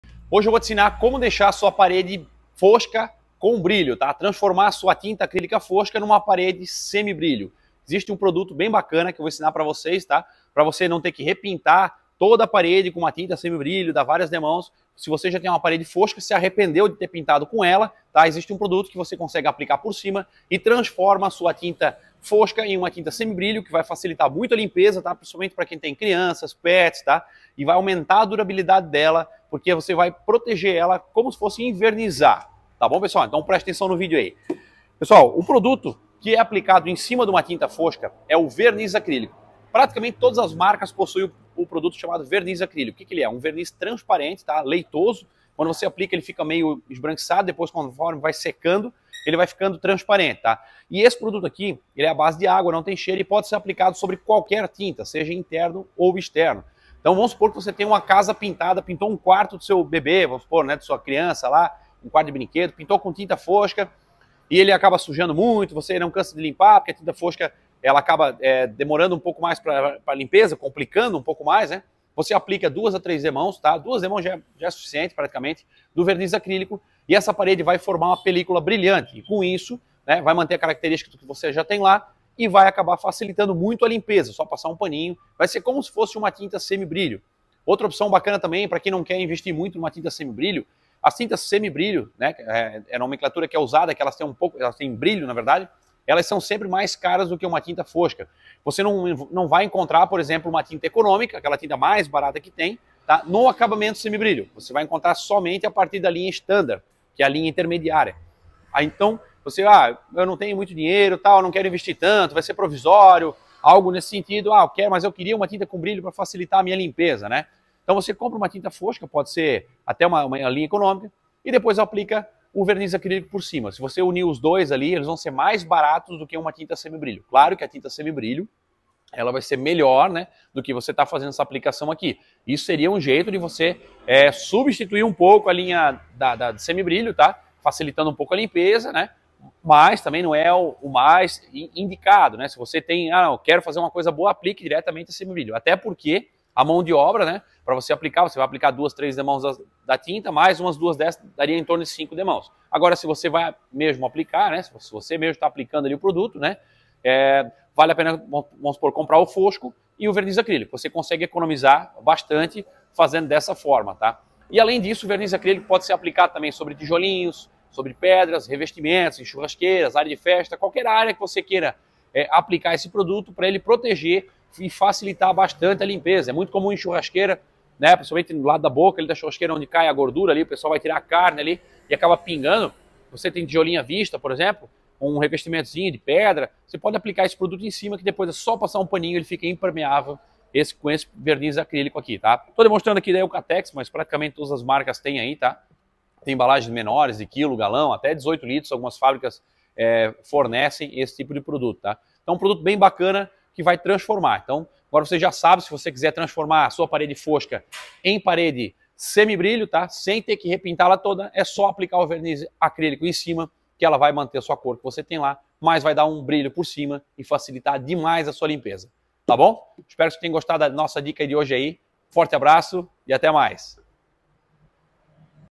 Como Transformar a Tinta Fosca em Semi Brilho, Conheça o Verniz Acrílico Impermeabilizante SAtintas
Hoje vamos falar sobre um produto chamado Verniz Acrilico, ideal para transformar tintas foscas em acabamento semi brilho para facilitar a sua limpeza